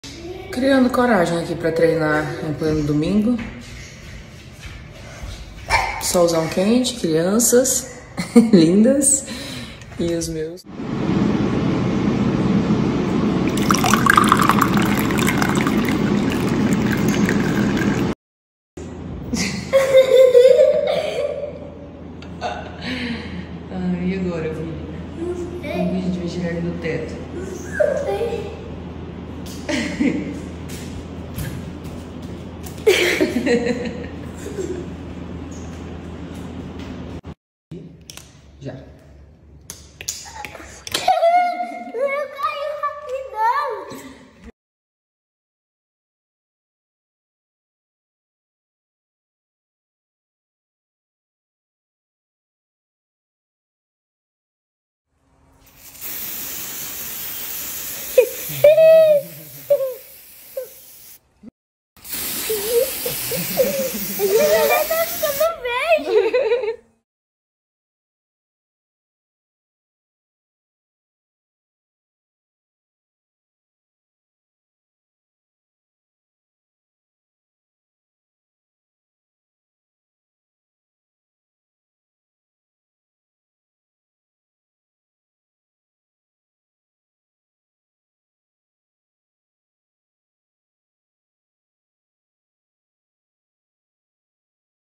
Gusttavo Lima e Andressa Suita curtem PISCINA na FAZENDA e se divertem com os filhos
Gusttavo Lima em dia de tranquilidade na fazenda junto com a esposa Andressa Suita e os filhos do casal, o Gabriel e o Samuel, se divertiram na piscina e a mamãe registrou momentos fofura